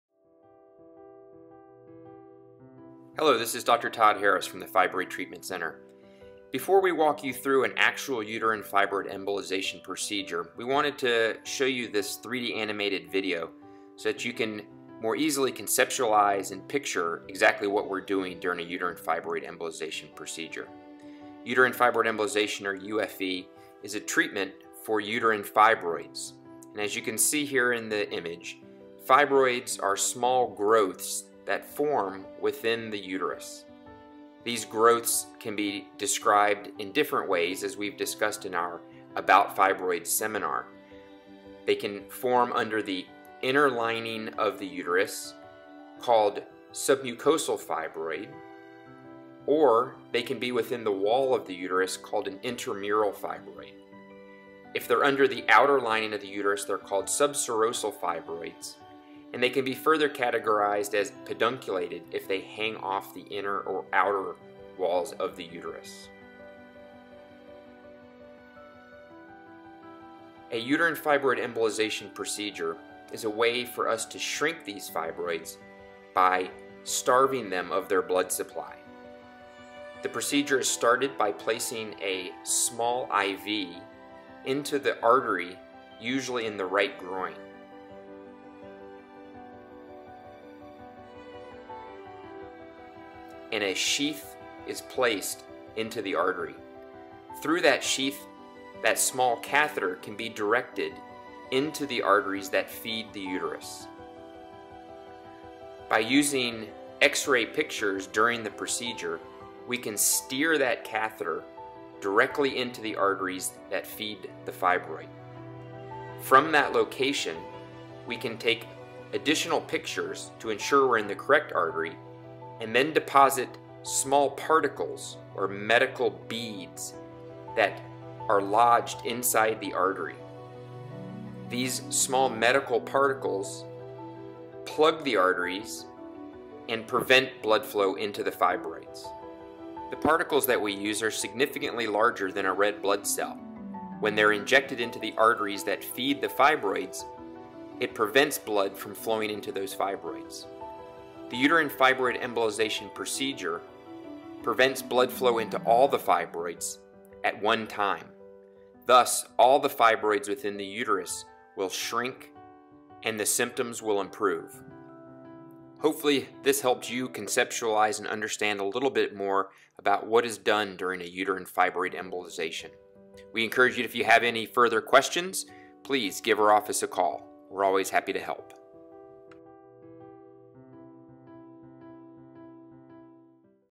Dr. Harris Narrates A 3-D Animation of a Uterine Fibroid Embolization (UFE)
Dr. Harris from The Fibroid Treatment Center narrates an easy to follow, 3-D animation which helps teach women what Uterine Fibroid Embolization entails.